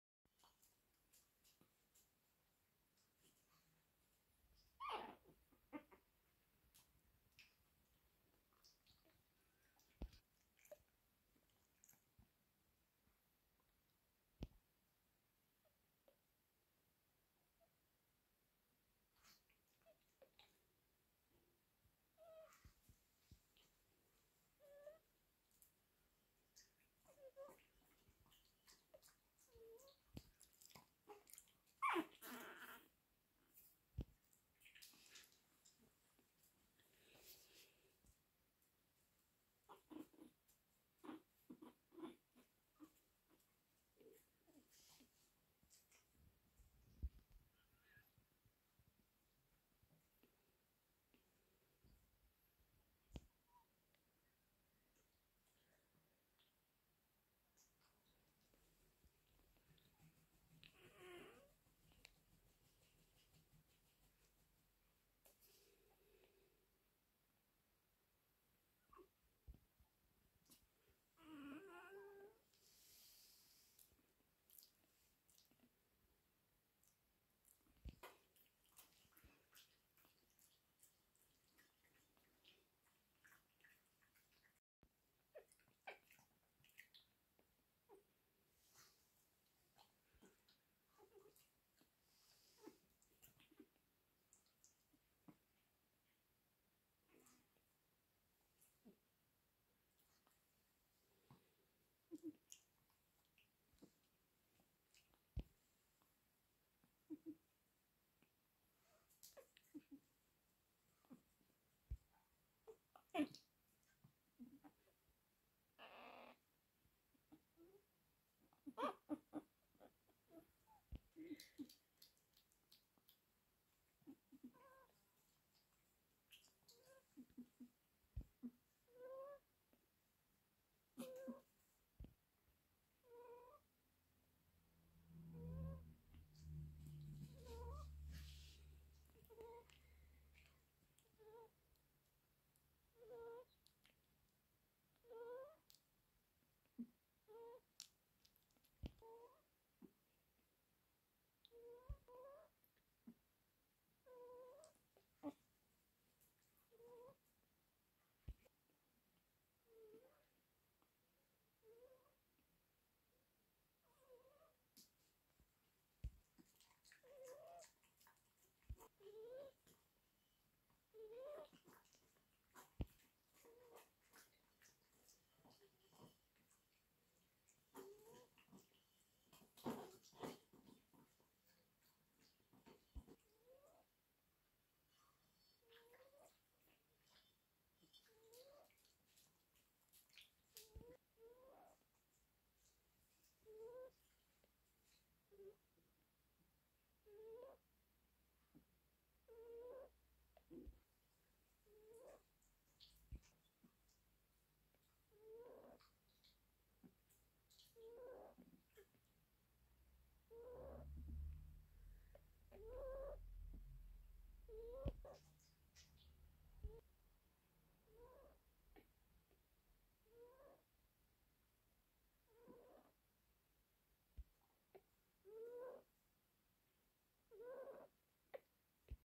Нашим ши-тцу 24 дні . Хто спить а хто хоче іграти @oksanapyrozhuk1385
Ши-тцу Віоли